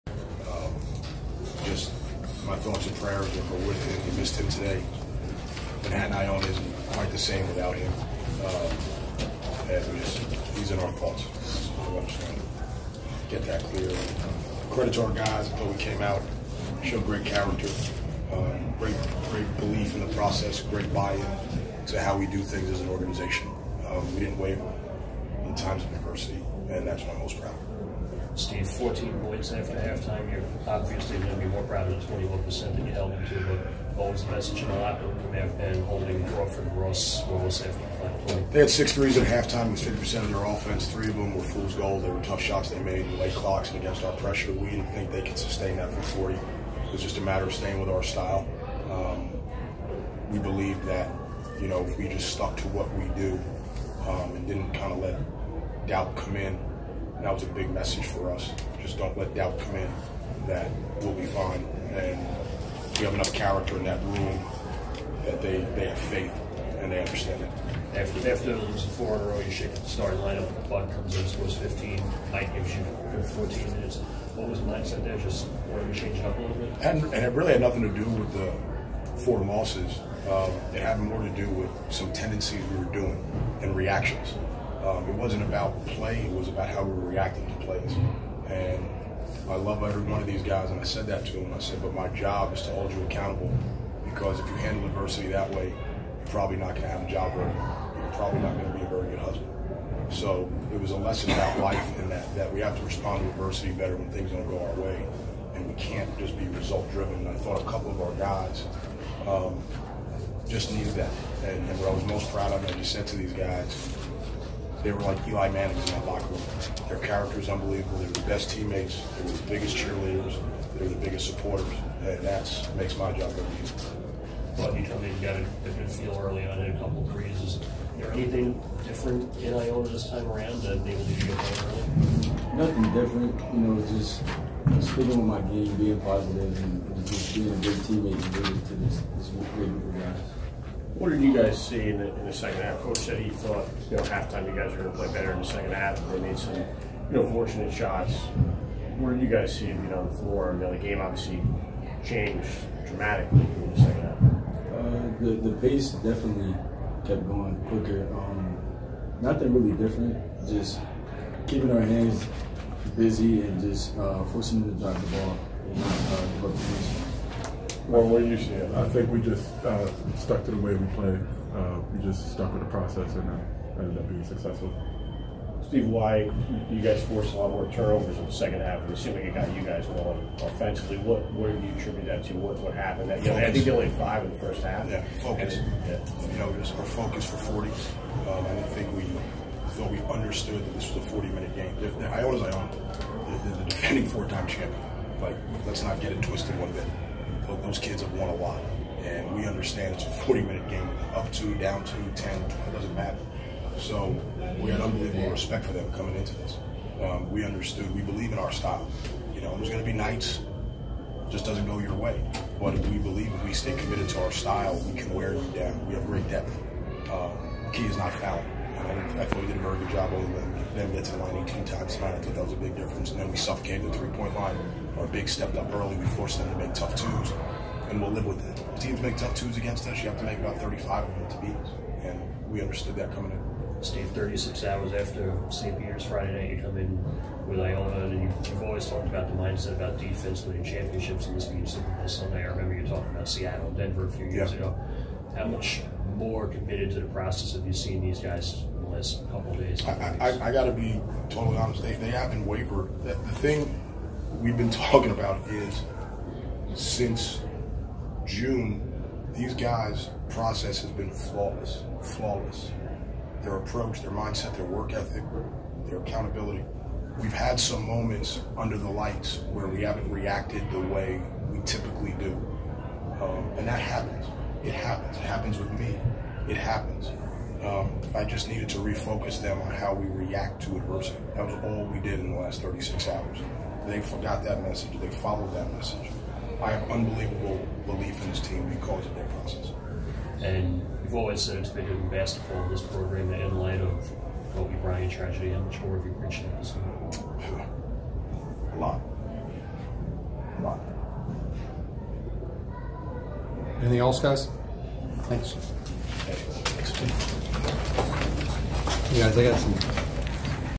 Manhattan Coach Steve Masiello, Players Post 72-49 Win Over Iona
Manhattan Coach Steve Masiello, Players Post 72-49 Win Over Iona. VIdeo by Ken Kraetzer for Sons of the American Legion Radio on Feb. 2, 2020